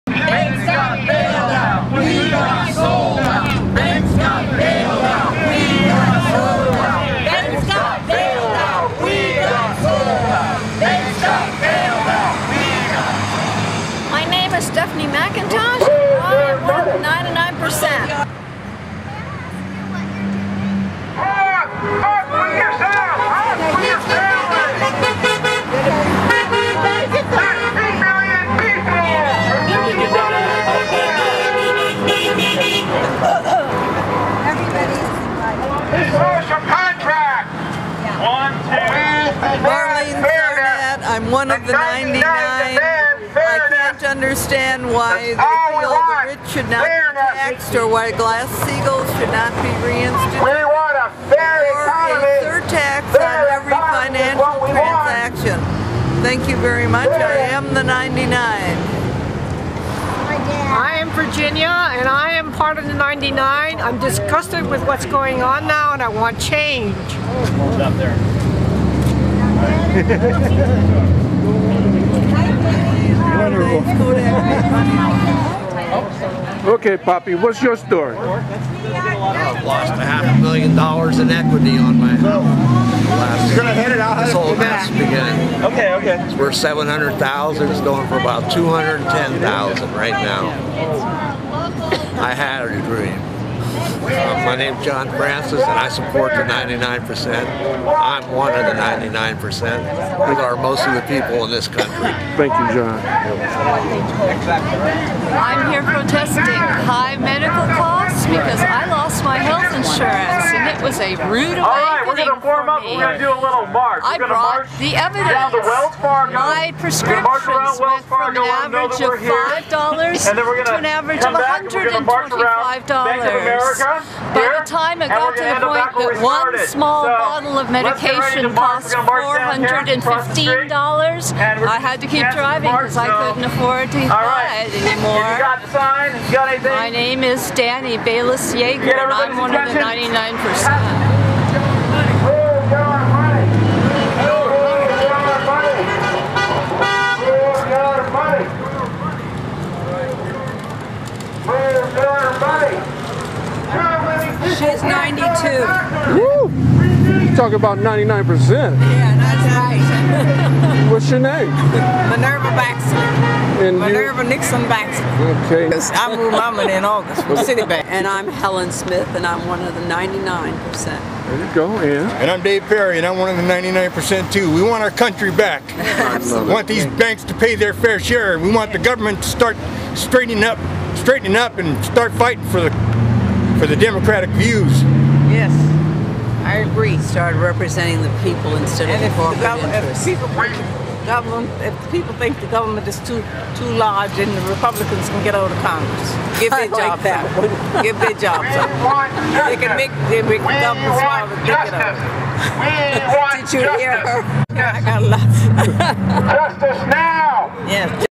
I Am The 99% "If the Government is too large..."
The title of this video comes from the words of 92 year old protester " Minerva Nixon Baxley

As part of the effort to bring attention to the inequities in income, the housing foreclosure crisis and high cost for health care people all over the country have come out to voice their concerns in peaceful protest.

On November 19, 2011 a small but vocal group of protesters marched in front of Wells Fargo and Bank of America in Antioch, CA. Among the group were many elderly who included men and women in the seventies, eighties and nineties!

From the reactions of those who drove by their is a great deal of support for the Occupy Wall Street movement and I suspect it will only gain in strength and numbers.